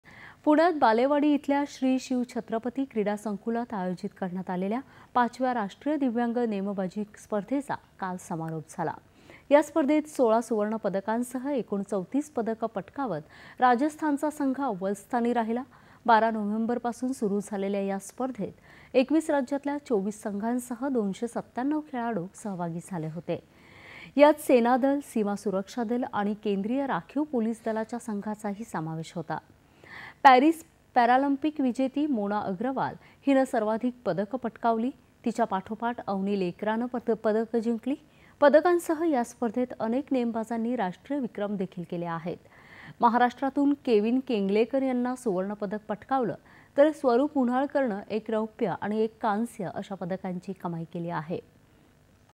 पुण्यात आयोजित करण्यात आलेल्या ५ व्या राष्ट्रीय दिव्यांग नेमबाजी स्पर्धेचा समारोप.
दि.१७.११.२०२४ :
             पुण्यात बालेवाडी इथल्या श्री शिव छत्रपती क्रीडा संकुलात आयोजित करण्यात आलेल्या ५ व्या राष्ट्रीय दिव्यांग नेमबाजी स्पर्धेचा काल समारोप झाला. या स्पर्धेत १६ सुवर्ण पदकांसह एकूण ३४ पदकं पटकावत राजस्थानचा संघ अव्वलस्थानी राहिला. १२ नोव्हेंबरपासून सुरू झालेल्या या स्पर्धेत २१ राज्यातल्या २४ संघांसह २९७ खेळाडू सहभागी झाले होते. यात सेनादल, सीमा सुरक्षा दल आणि केंद्रीय राखीव पोलीस दलाच्या संघांचाही समावेश होता.  पॅरिस पॅरालिम्पिक विजेती मोना अग्रवाल हिनं  सर्वाधिक पदकं पटकावली. तिच्या पाठोपाठ अवनी लेकरा नं पदकं  जिंकली. पदकांसह या स्पर्धेत अनेक नेमबाजांनी राष्ट्रीय विक्रम देखील केले.  महाराष्ट्रातून केविन केंगलेकर यानं सुवर्णपदक पटकावलं, तर स्वरूप उन्हाळकरनं एक रौप्य आणि एक कांस्य अशा पदकांची कमाई केली.

8.30am | 11.00am | 1.00pm | 4.30pm | 7.00pm | 9.30pm